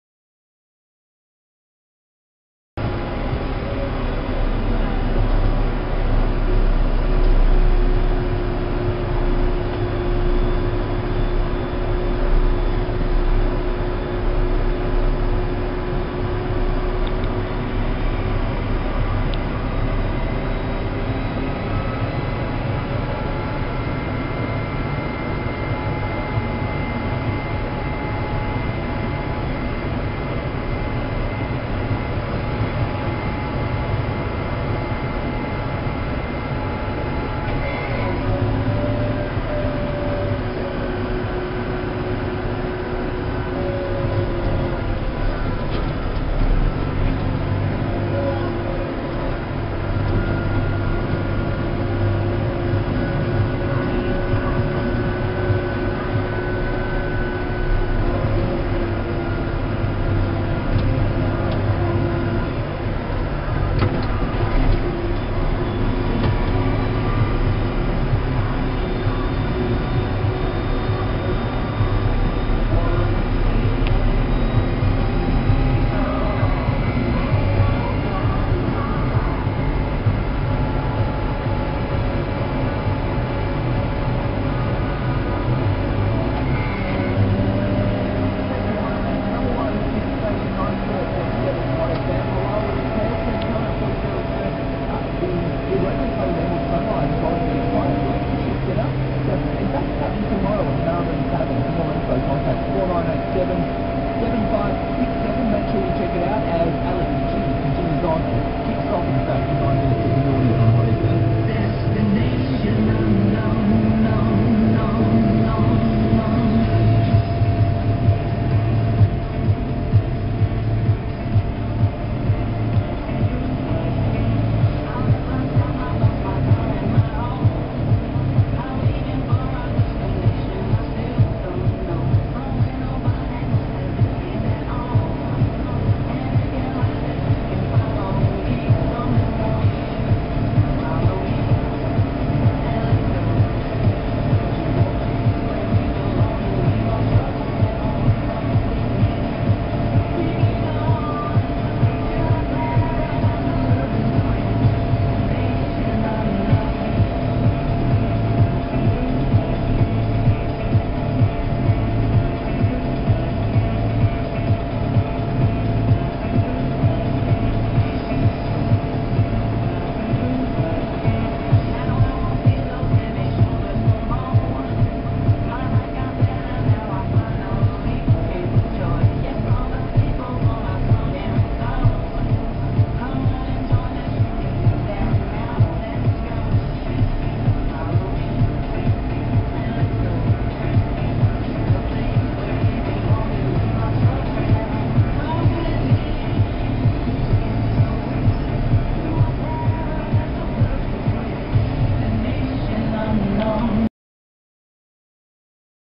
Roadworks while moving on the highway, Capella to Clermont
Nothing much, just going through some roadworks between Capella and Clermont, Central Highlands, Queensland.